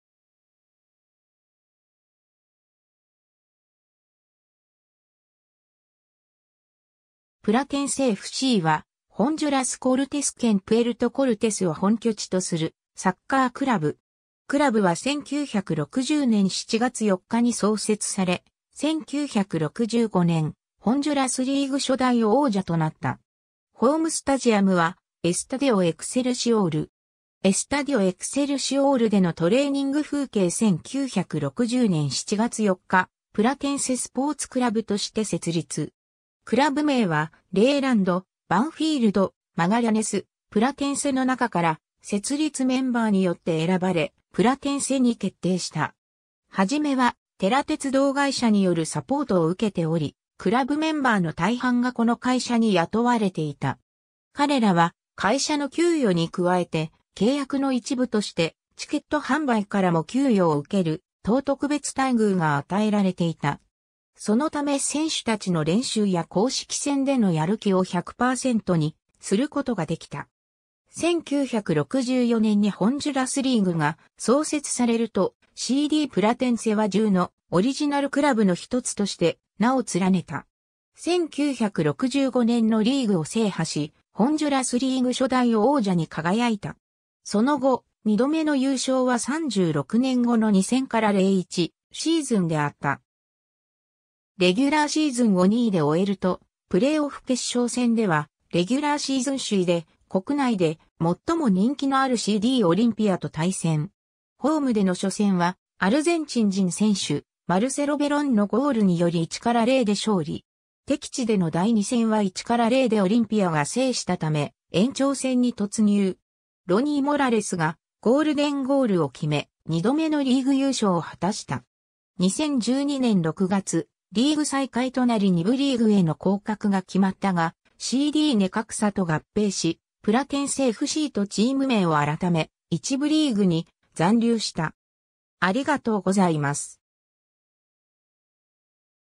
プラテンセFC
プラテンセFC（Platense Fútbol Club）は、ホンジュラス・コルテス県プエルト・コルテスを本拠地とするサッカークラブ。
 クラブは1960年7月4日に創設され、1965年、ホンジュラスリーグ初代王者となった。
ホームスタジアムはエスタディオ・エクセルシオール。
 エスタディオ・エクセルシオールでのトレーニング風景（2005年） 1960年7月4日、プラテンセスポーツクラブとして設立。
クラブ名はレイランド、バンフィールド、マガリャネス、プラテンセの中から設立メンバーによって選ばれ、プラテンセに決定した。
始めはテラ鉄道会社によるサポートを受けており、クラブメンバーの大半がこの会社に雇われていた。
彼らは会社の給与に加えて、契約の一部としてチケット販売からも給与を受ける等特別待遇が与えられていた。
その為選手たちの練習や公式戦でのやる気を100%にすることが出来た。
 1964年にホンジュラスリーグが創設されると、CDプラテンセは10のオリジナルクラブの一つとして名を連ねた。
 1965年のリーグを制覇し、ホンジュラスリーグ初代王者に輝いた。
その後、2度目の優勝は36年後の2000-01（C）シーズンであった。
レギュラーシーズンを2位で終えると、プレーオフ決勝戦ではレギュラーシーズン首位で、国内で最も人気のあるCDオリンピアと対戦。
ホームでの初戦はアルゼンチン人選手・マルセロ・ベロンのゴールにより1-0で勝利。
敵地での第二戦は1-0でオリンピアが制した為延長戦に突入。
ロニー・モラレスがゴールデンゴールを決め、2度目のリーグ優勝を果たした。
 2012年6月、リーグ最下位となり2部リーグへの降格が決まったが、CDネカクサと合併しプラテンセFCとチーム名を改め、1部リーグに残留した。
 (1994–現在)